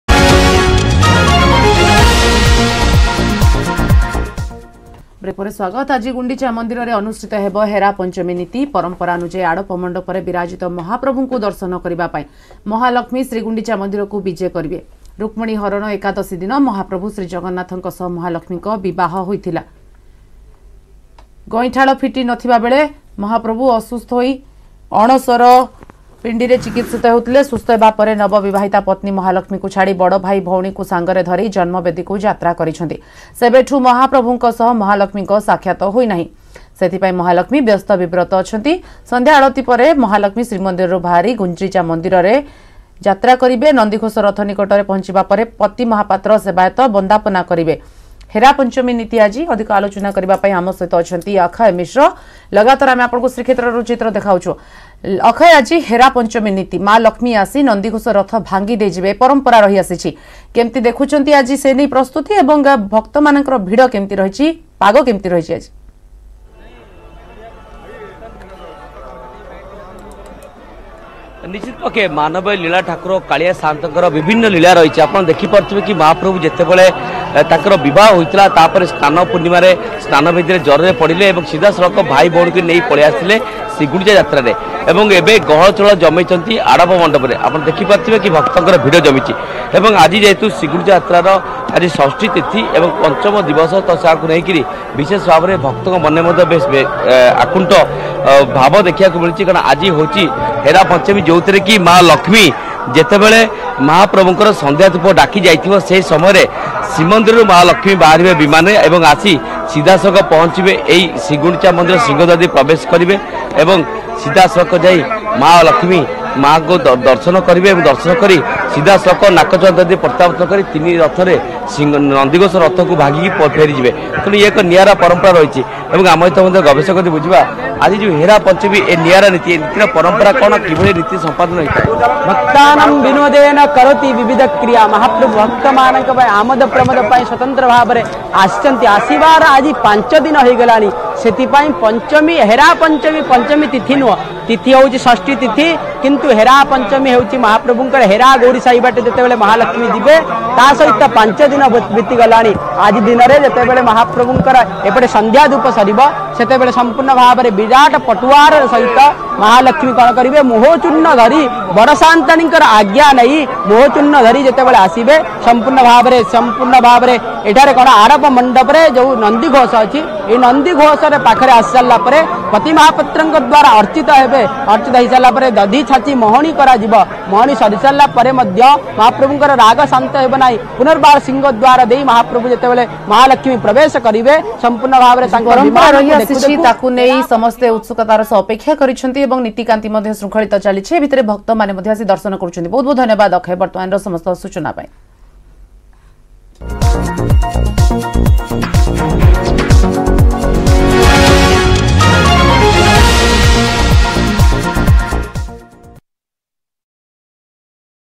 HeraPanchami | Puri | ମହାପ୍ରଭୁଙ୍କ ହେରାପଞ୍ଚମୀ ନୀତି , ଶ୍ରୀମନ୍ଦିରରେ କେମିତି ରହିଛି ପ୍ରସ୍ତୁତି ?
HeraPanchami | Puri | ମହାପ୍ରଭୁଙ୍କ ହେରାପଞ୍ଚମୀ ନୀତି , ଶ୍ରୀମନ୍ଦିରରେ କେମିତି ରହିଛି ପ୍ରସ୍ତୁତି | ଗୁଣ୍ଡିଚା ମନ୍ଦିରରେ ହେବ ହେରାପଞ୍ଚମୀ ନୀତି , କେନ୍ଦ୍ରାପଡାରେ ପ୍ରସ୍ତୁତି ଶେଷ  | ମହାପ୍ରଭୁଙ୍କ  ଦର୍ଶନ ନ ପାଇ ରଥ ଭାଙ୍ଗିବେ ମହାଲକ୍ଷ୍ମୀ |  News18 Odia